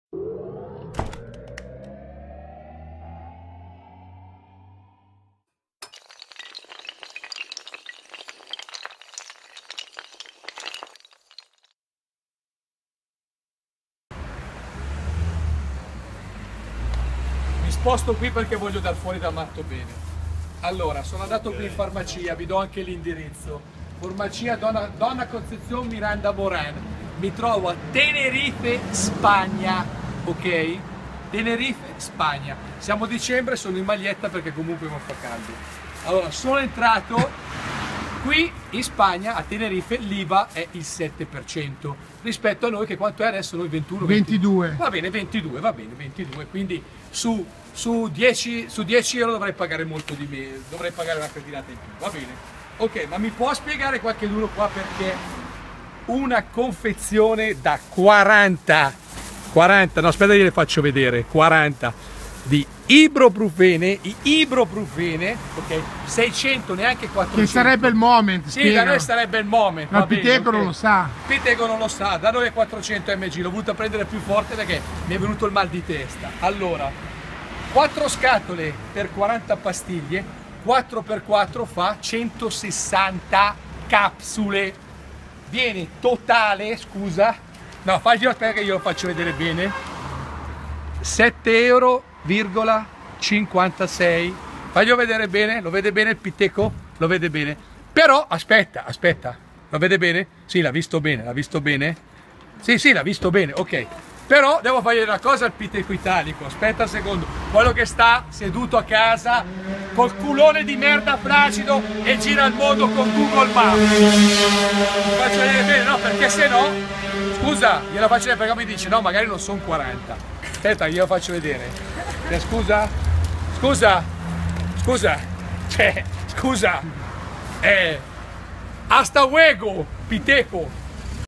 PREZZI FARMACI a TENERIFE (spagna) !!!
Lambrenedetto vi fa vedere i prezzi dei farmaci a Tenerife guardate che roba !!!